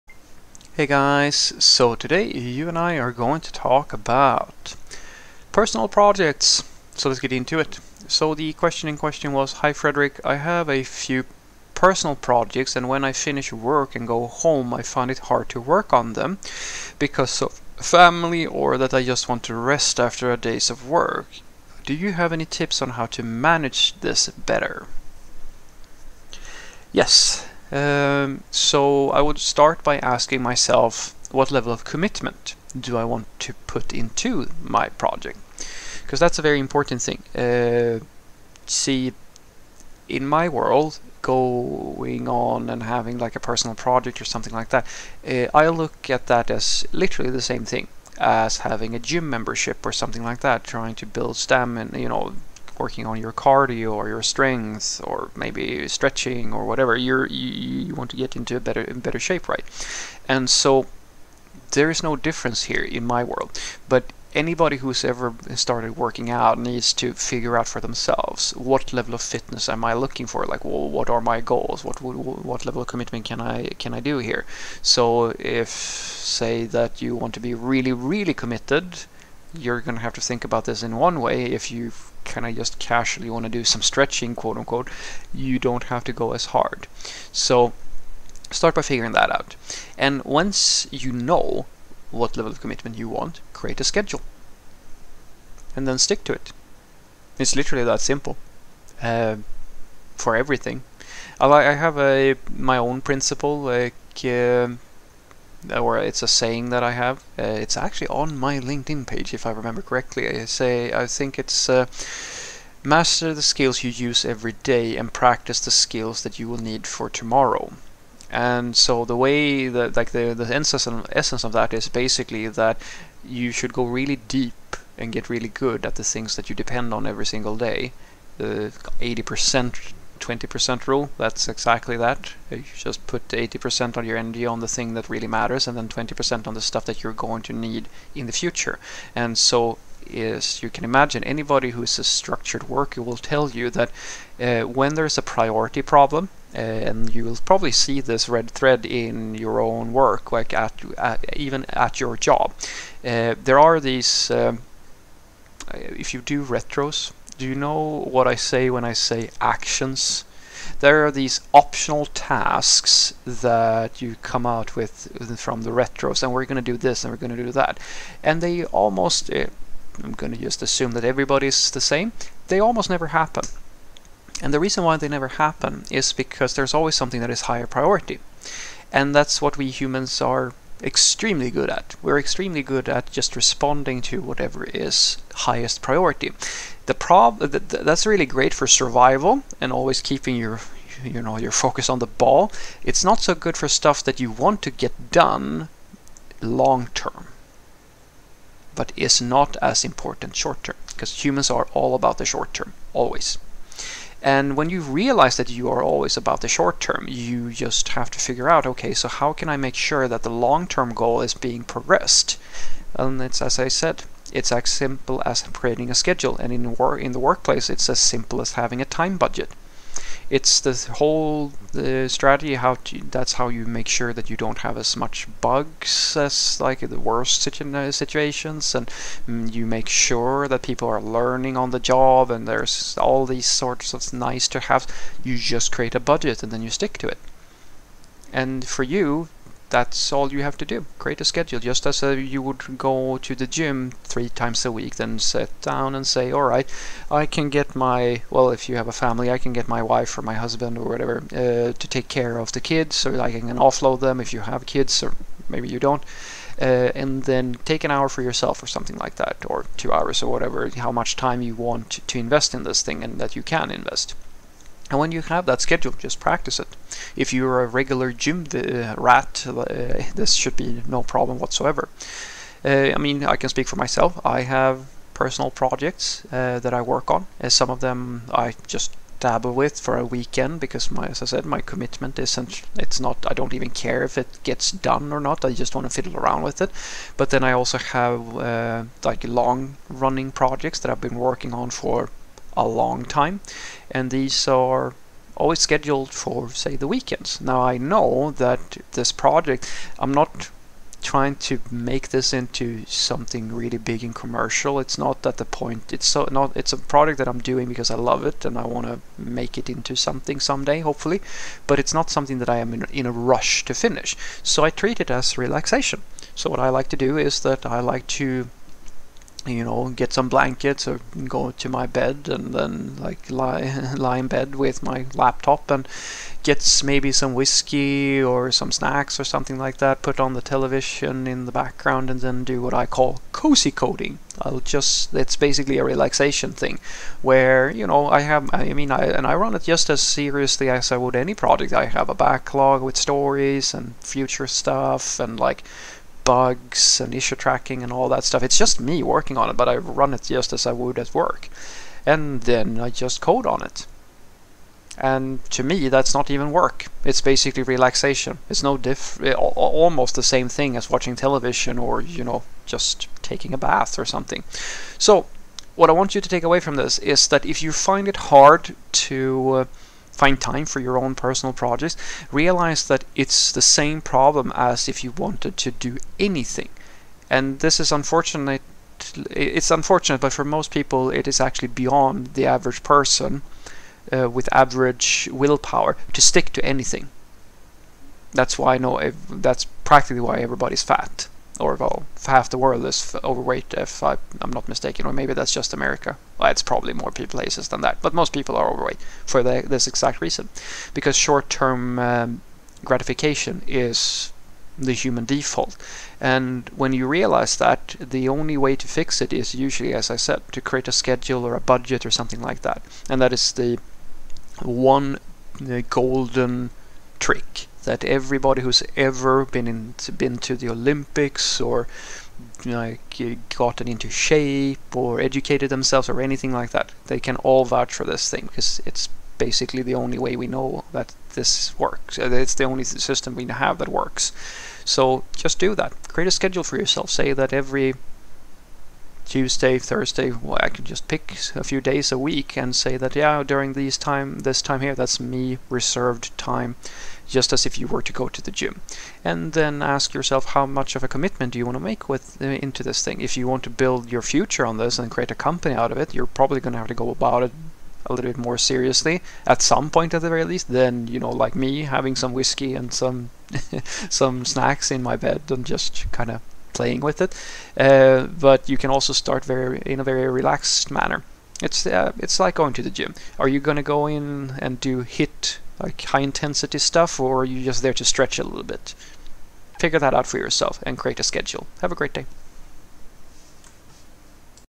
What if I have a hard time to commit to my side projects?
In "What if I have a hard time to commit to my side projects?" I answer a programming question.